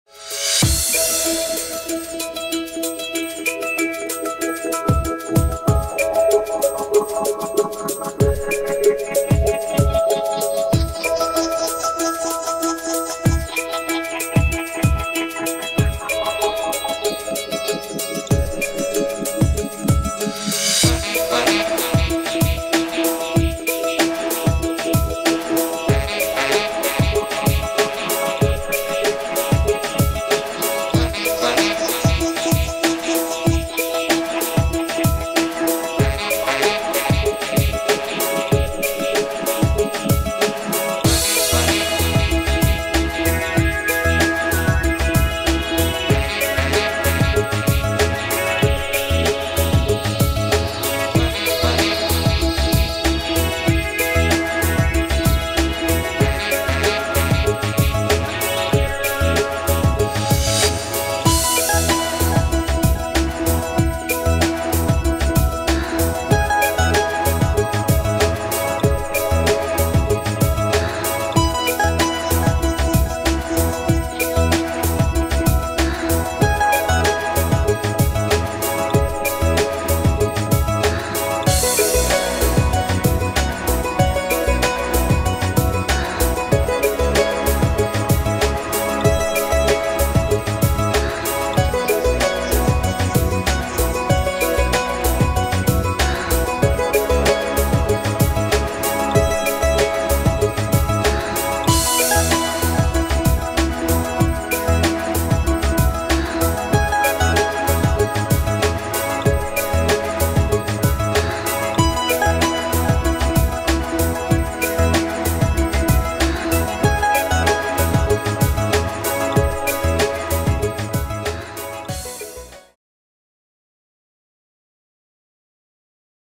Restaurante Salta Argentina José Balcacer Cocina Fusión Andina 2016
La mejor comida Fusión Andina José Balcarce Salta - Argentina. Disfruten de esta producción al igual que yo lo hice, Gracias por la confianza  .